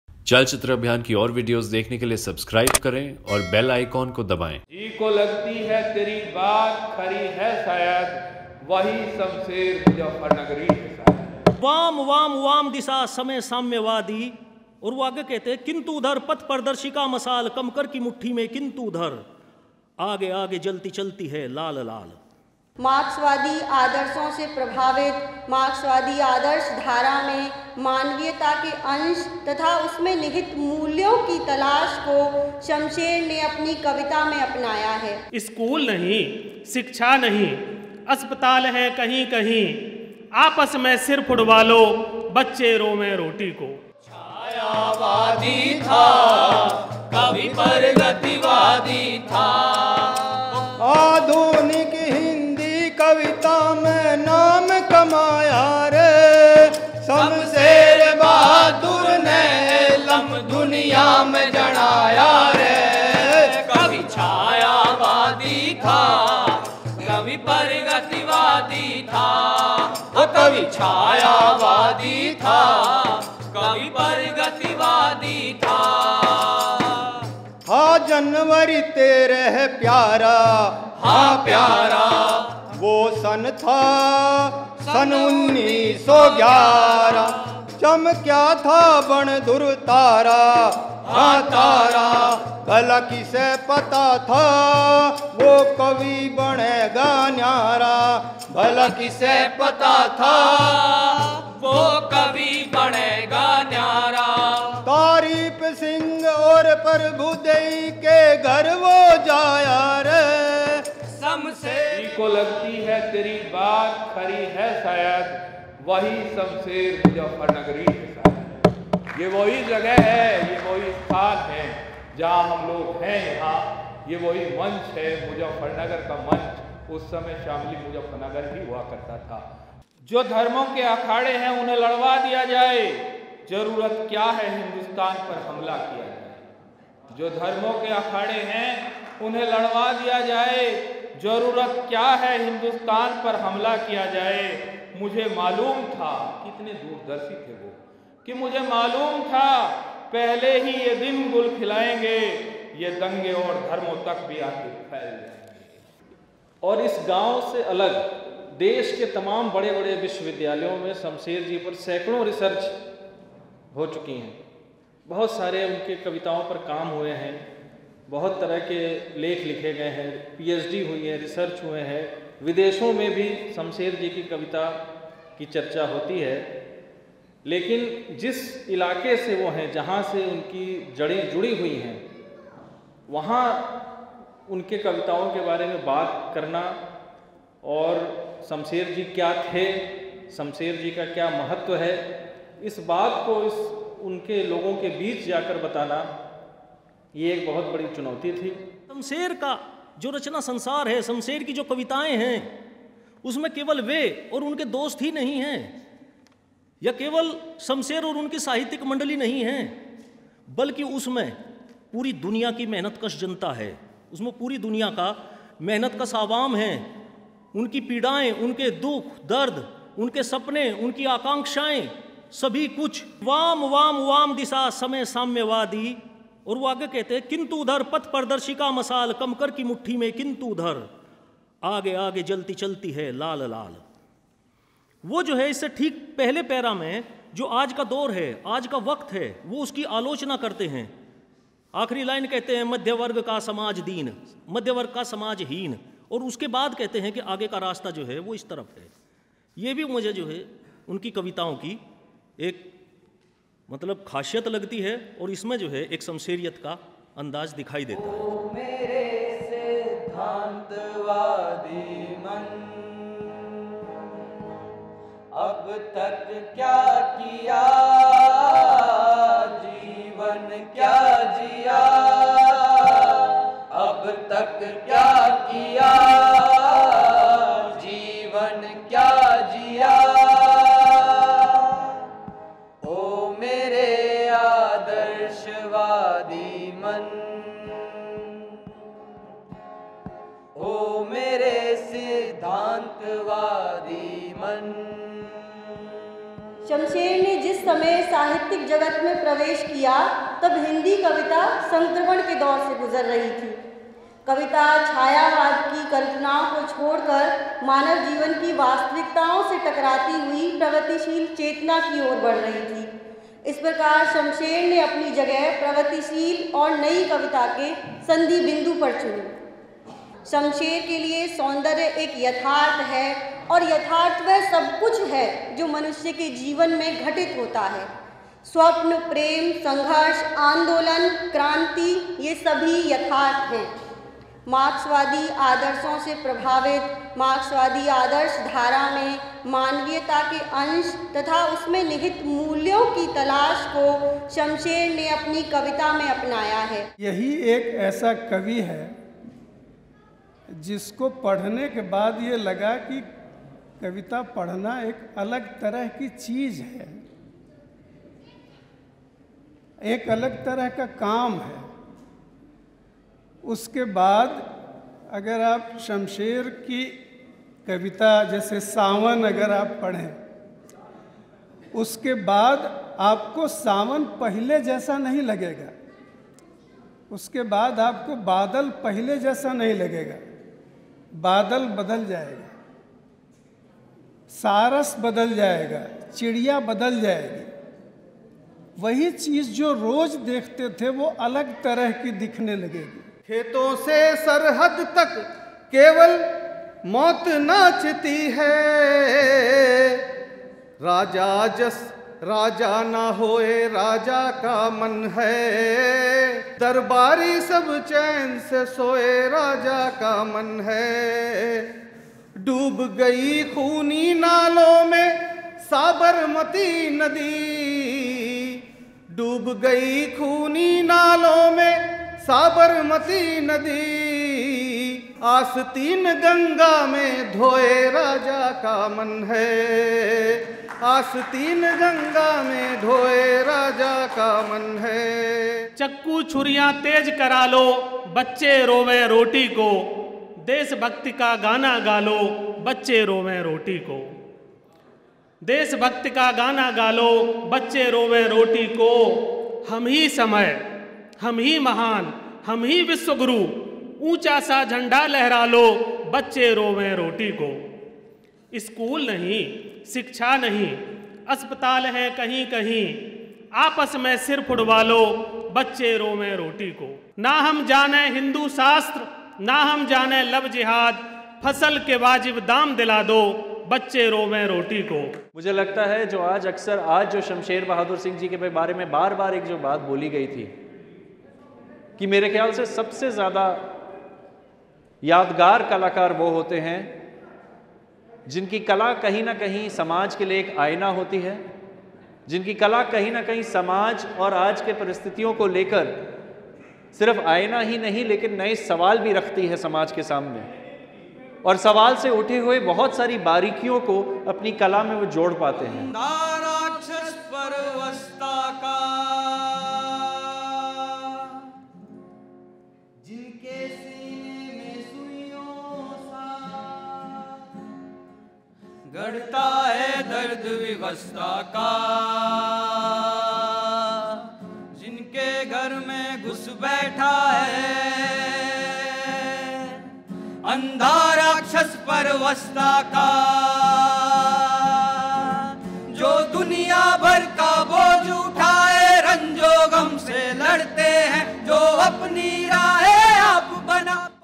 शामली: शमशेर बहादुर सिंह की जयंती पर जन सैलाब| Shamsher Bahadur Singh's jayanti stir emotions- CCA
उत्तर प्रदेश ज़िला शामली के गांव एलम में दिनाक 14 जनवरी 2024 को नौजवानों और ग्रामीणों के साथ मिलकर प्रसिद्ध प्रगतिशील कवि शमशेर बहादुर सिंह की जयंती पर  'अमन के राग' के रूप में युवा संवाद किया । इस कार्यक्रम को अलग-अलग वक्ताओं ने कवि शमशेर बहादुर सिंह पर अपनी बात रखी। चलचित्र अभियान और नौजवान भारत सभा व जसम संगठनो ने साथ आकर कार्यक्रम में भाग लिया ।

On January 14, 2024, the village of Elam in Shamli district, Uttar Pradesh, witnessed a gathering of youth and villagers coming together to celebrate the birth anniversary of the esteemed and forward-thinking poet, Shamsher Bahadur Singh. The event, themed 'Melody of Peace,' held discussions about the poet at various times. The celebration was attended by young people, and villagers, and was jointly organised by ChalChitra Abhiyaan, Bharat Naujawan Sabha, and Jasam.

Crew -Mohd Savez Chauhan, Shakib Yameen Rangrezz, Vishal Stonewall

CCA ground report